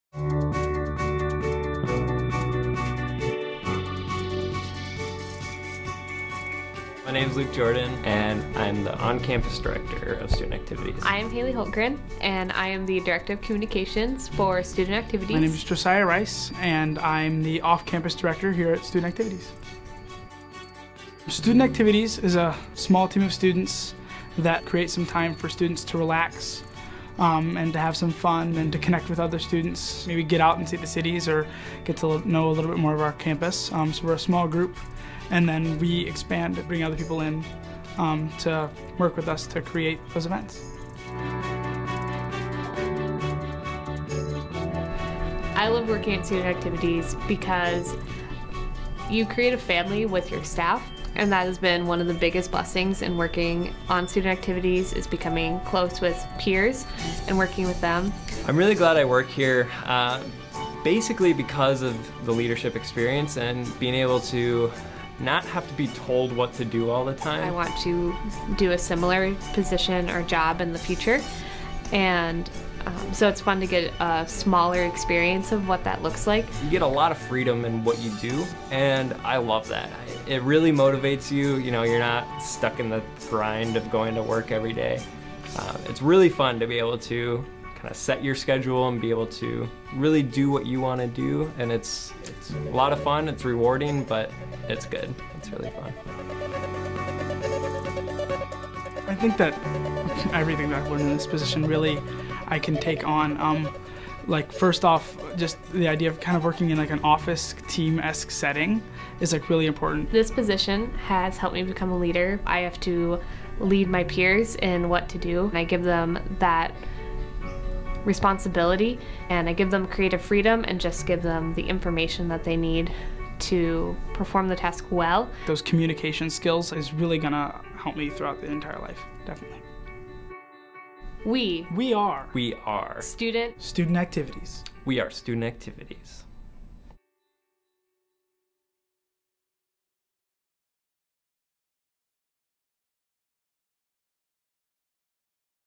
We are Student Activities--Northwestern
This video is a documentary and promotional video about the Student Activities organization at the University of Northwestern St. Paul.